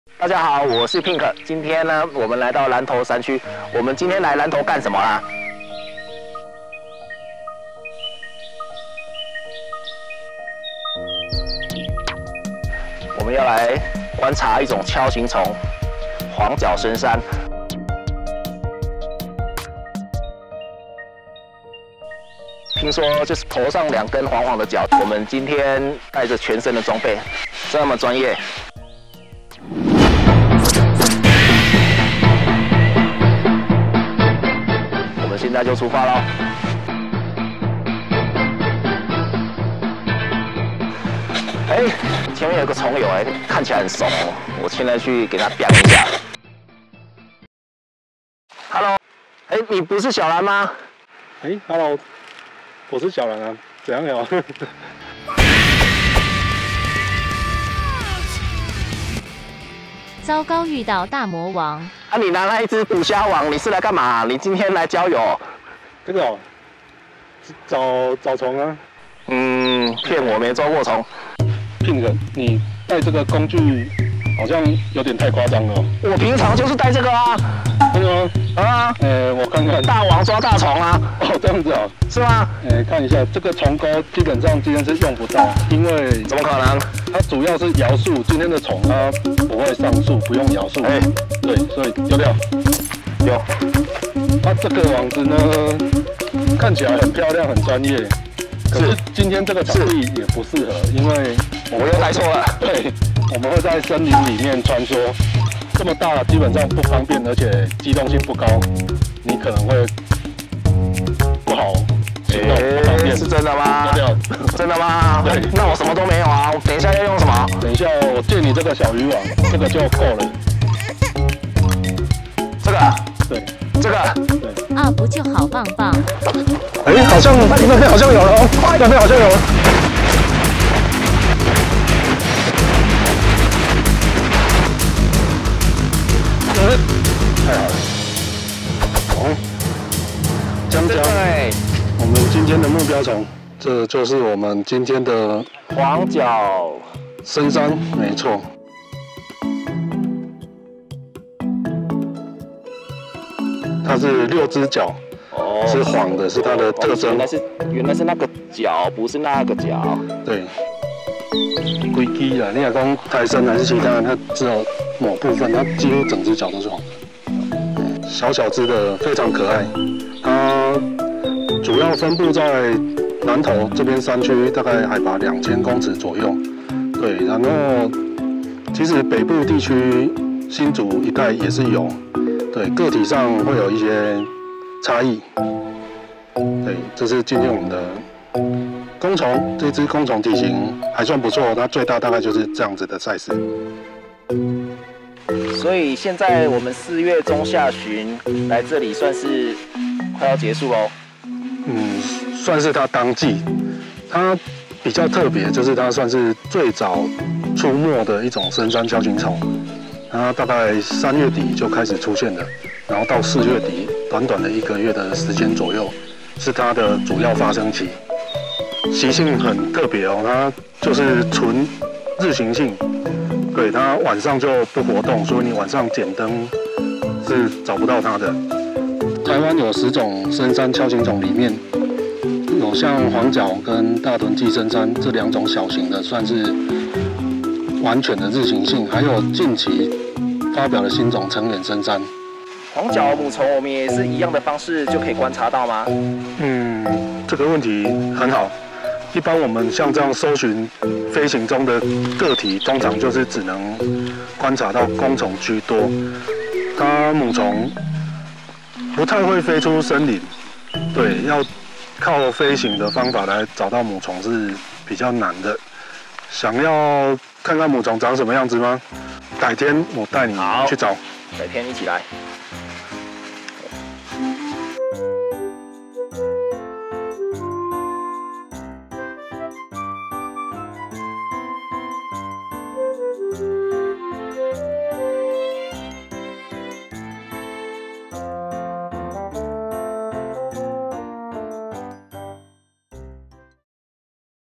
【跟著小粉小藍找虫虫】黃腳深山鍬形蟲(第一集)
一起上山找黃腳深山囉!
這一集咱們一起了解公蟲的習性